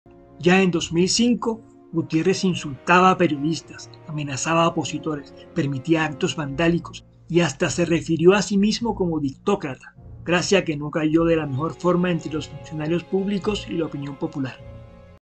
Recordando al DICTOCRATA Lucio GUTIÉRREZ | La caída de otro PRESIDENTE en ECUADOR
En 2005 las acciones de Lucio Gutiérrez en la presidencia de la Republica demostraban una clara intención de ejercer su mandato con acciones cercanas a la dictadura. Un pueblo aguerrido, y acostumbrado a reaccionar de forma enérgica ante los malos manejos de los mandatarios, alzo su voz en contra del Ejecutivo y logro sacar del Carondelet a este simpático pero nefasto y peligroso personaje.